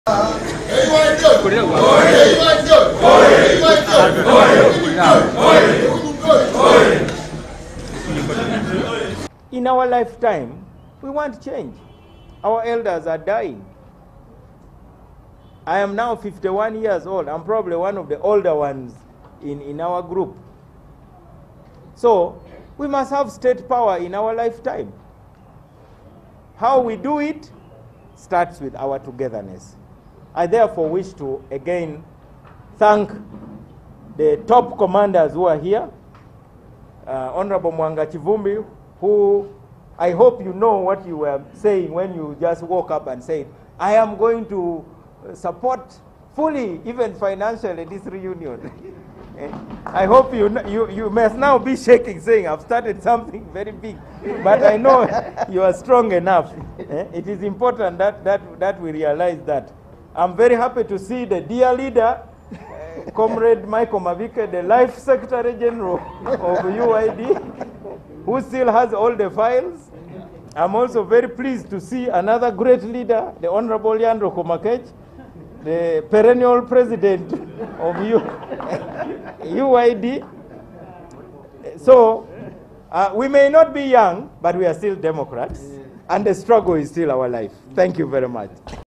Kivumbi, Mabikke, Lubega Mukaaku return to DP
A number of politicians who had abandoned the Democratic Party have returned to the fold. Among them are MP Muwanga Kivumbi, Michael Mabikke who had formed the Social Democratic Party, Samuel Lubega Mukaaku- a one-time presidential candidate who announced their return to the party headed by Norbert Mao. They held a press conference in Kampala.